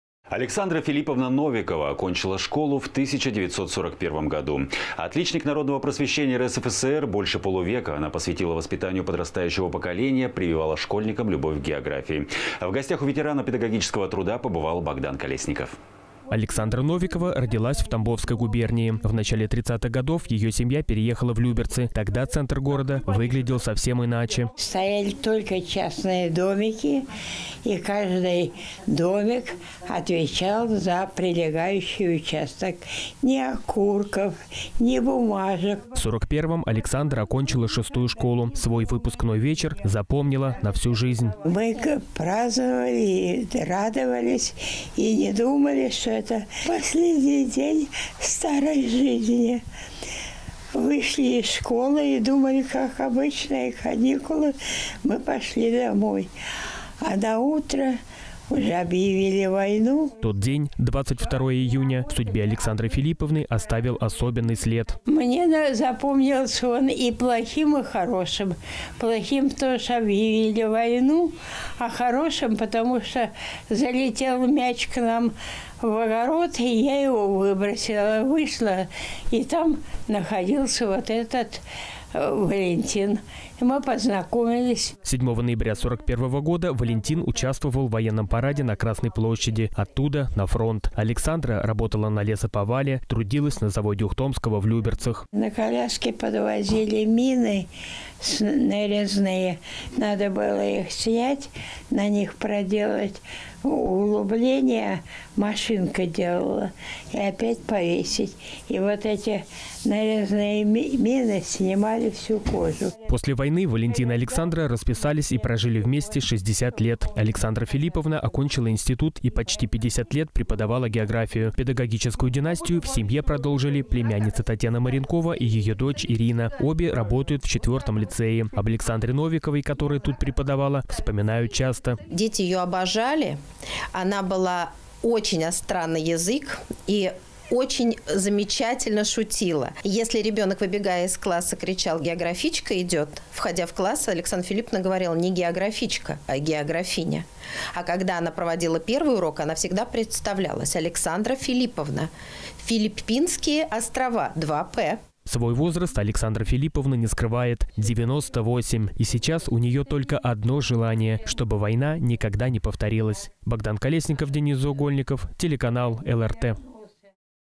Воспоминания выпускницы 1941 года
Александра Филипповна Новикова окончила школу в 1941 году. Отличник народного просвещения РСФСР, больше полувека она посвятила воспитанию подрастающего поколения, прививала школьникам любовь к географии. В гостях у ветерана педагогического труда побывали мои коллеги.

Александра Новикова родилась в Тамбовской губернии. В начале тридцатых годов её семья переехала в Люберцы. Тогда центр города выглядел совсем иначе.

Александра Новикова, ветеран Великой Отечественной войны: «Стояли только частные домики. И каждый домик отвечал за прилегающий участок. Ни окурков, ни бумажек».

В сорок первом Александра окончила 6-ю люберецкую школу. Свой выпускной вечер запомнила на всю жизнь.

Александра Новикова, ветеран Великой Отечественной войны: «Мы праздновали, радовались и не думали, что это последний день старой жизни. Вышли из школы и думали, что как обычные каникулы, и мы пошли домой. А на утро уже объявили войну».

Тот день – 22 июня – в судьбе Александры Филипповны оставил особенный след.

Александра Новикова, ветеран Великой Отечественной войны: «Мне запомнился он и плохим, и хорошим. Плохим, потому что объявили войну. А хорошим – потому что залетел мяч к нам в огород, и я его выбросила. Вышла, и там находился Валентин. И мы познакомились».

7 ноября 41-го года Валентин участвовал в военном параде на Красной площади. Оттуда –  на фронт. Александра работала на лесоповале, трудилась на заводе Ухтомского в Люберцах.

Александра Новикова, ветеран Великой Отечественной войны: «На коляске подвозили мины нарезные. Надо было их снять, на них проделать углубление машинкой и опять повесить. И вот эти нарезные мины снимали всю кожу».

После войны Валентин и Александра расписались и прожили вместе 60 лет. Александра Филипповна окончила институт и почти 50 лет преподавала географию. Педагогическую династию в семье продолжили племянница Татьяна Маренкова и её дочь Ирина. Обе работают в 4-м лицее. Об Александре Новиковой, которая тут преподавала, вспоминают часто.

Ирина Маренкова, директор лицея № 4 г.о. Люберцы, внучатая племянница А.Ф. Новиковой: «Дети её обожали. Она была очень остра на язык и очень замечательно шутила. Если ребёнок, выбегая из класса, кричал: «Географичка идёт», входя в класс, Александра Филипповна говорила: «Не географичка, а географиня». А когда она проводила первый урок, она всегда представлялась: «Александра Филипповна. Филиппинские острова. Два «п»».

Свой возраст Александра Филипповна не скрывает. 98! И сейчас у неё только одно желание: чтобы война никогда не повторилась.